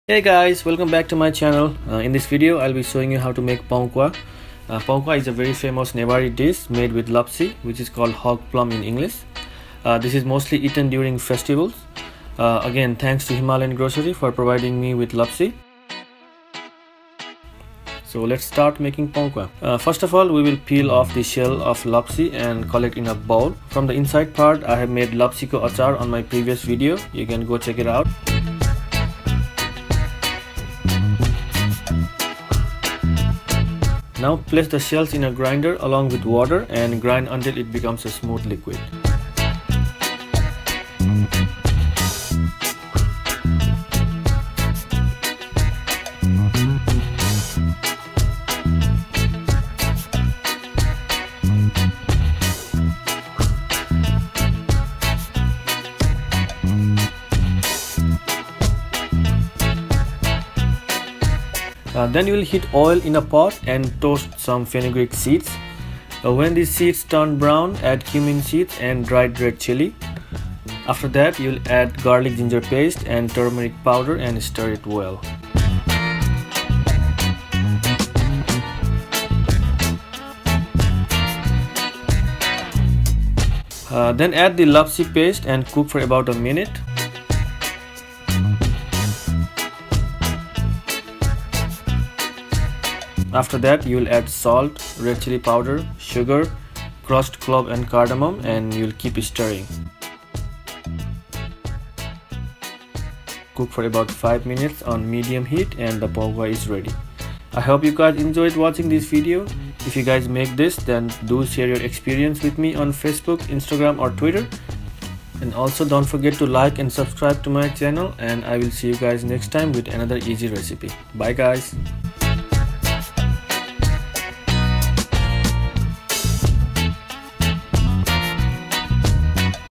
Pau quaa | Nepali Food Recipe | Anup Kitchen
Pau quaa is a Newari delicacy made with lapsi (Hog Plum). It is sweet and sour soupy dish eaten during festivals.

Ingredients:

Lapsi (hog plum): 800g
Fenugreek seeds: 1/2 tsp
Dried red chili: 3
Cumin seeds: 1/2 tsp
Garlic Ginger paste: 1 tsp
Turmeric powder: 1/4 tsp
Salt: 1tsp
Red chili powder: 1/2 tsp
Sugar: 5 tbsp
Cardamom: 3
Cloves: 3
Oil:  2 tbsp
Water: 2 cup